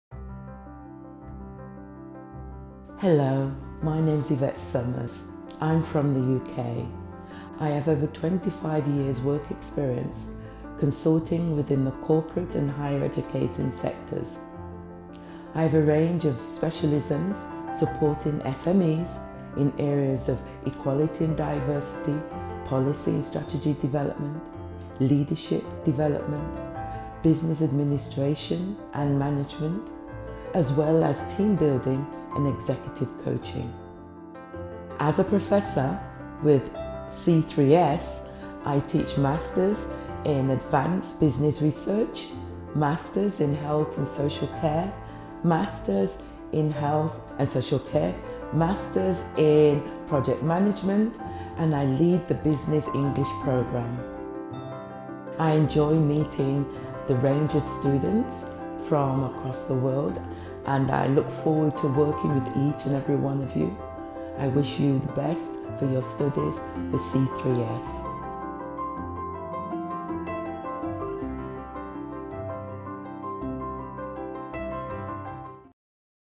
Professor Yvette Summers | C3S Business School
Yvette Summers, a Business English professor at C3S Business School, shares her valuable insights about the school, its campus, teaching methodology amongst other things. Our academic team comprises industry experienced experts who share their experiences to shape the students for a better tomorrow.